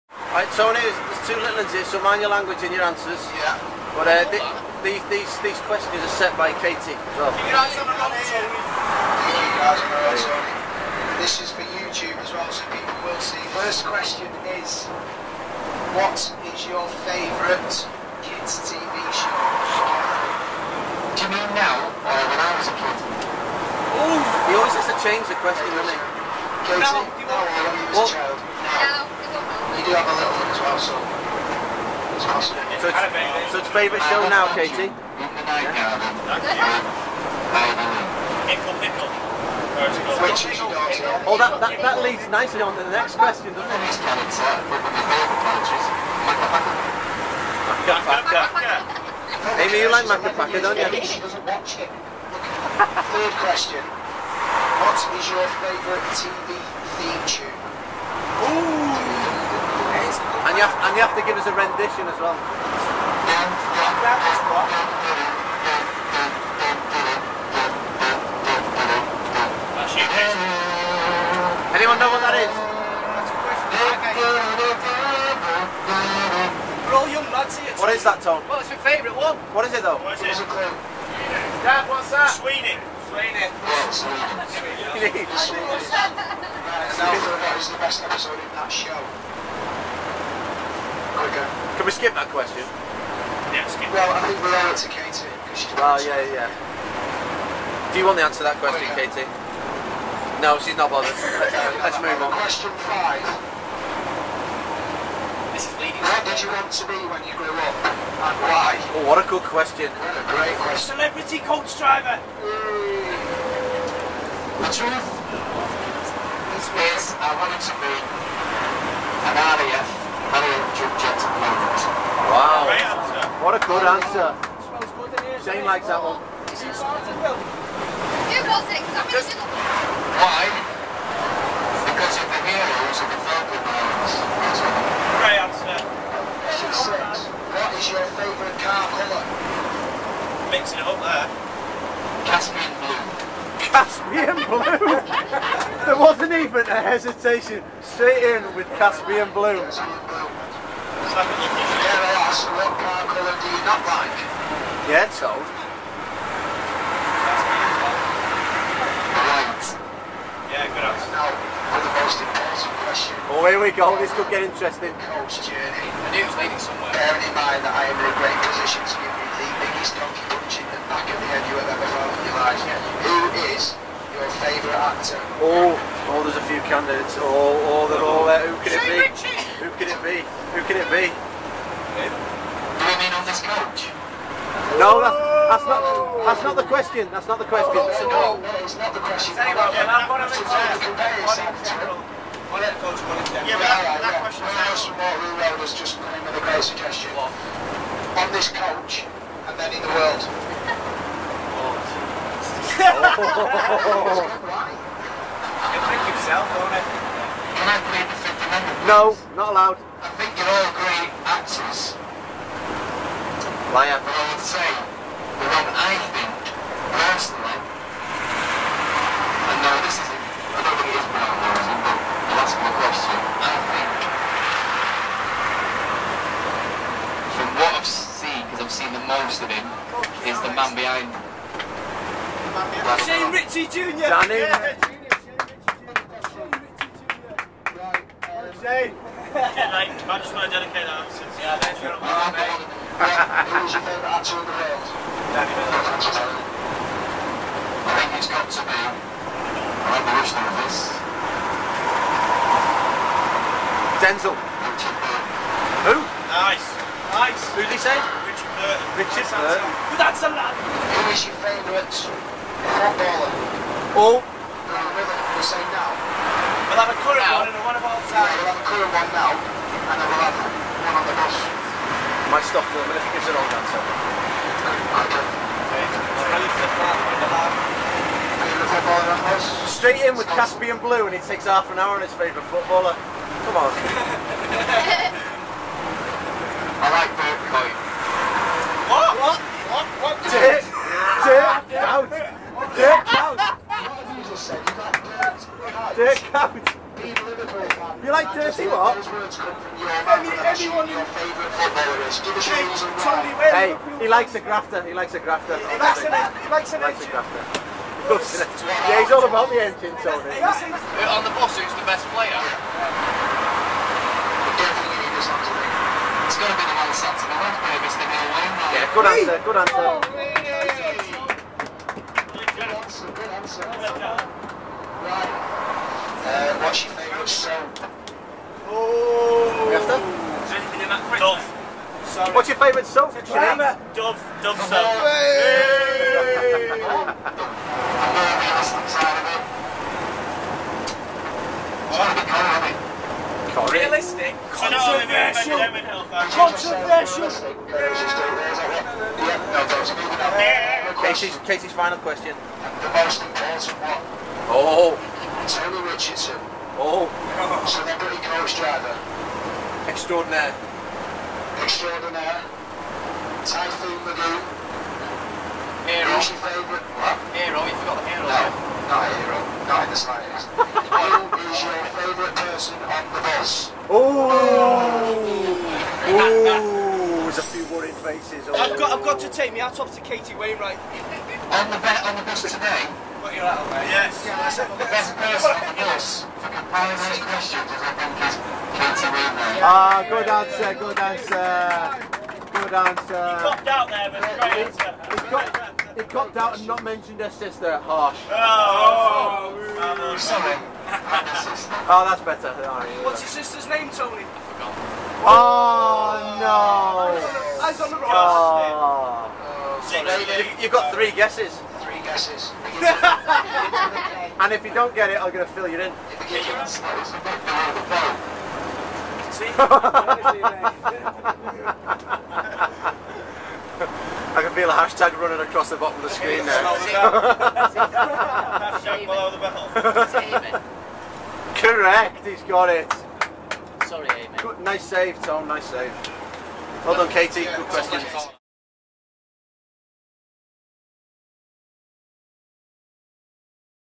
Tony The Coach Driver Questioned By 11 Year Old Katy (Read by Danny Miller)
Tony answers questions set by 11 year old Katy Wainwright, read out on the mic by Danny Miller...